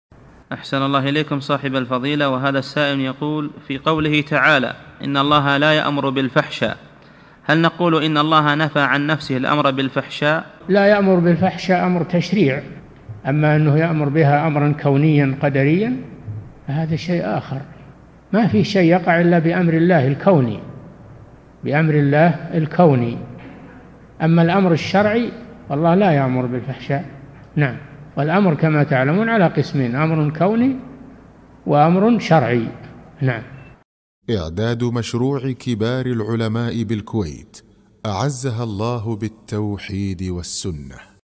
[36 -858] في قوله تعالى: (إن الله لا يأمر بالفحشاء) هل نقول إن الله نفى عن نفسه الأمر بالفحشاء؟
-"السؤال: في قوله تعالى: (إن الله لا يأمر بالفحشاء) هل نقول إن الله نفى عن نفسه الأمر بالفحشاء؟
الجَوَابُ: لا يأمر بالفحشاء أمر تشريع، أما أنه يأمر بها أمرا كونياً قدرياً فهذا شيء آخر، ما في شيء يقع إلا بأمر الله الكوني بأمر الله الكوني، أما الأمر الشرعي الله لا يأمر بالفحشاء، نعم. والأمر كما تعلمون على قسمين، أمر كوني، وأمر شرعي.
للاشتراك في قناة الشيخ صالح الفوزان

للاستفسار والإقتراحات والدعم
رئيس الفريق
لمشروع كبار العلماء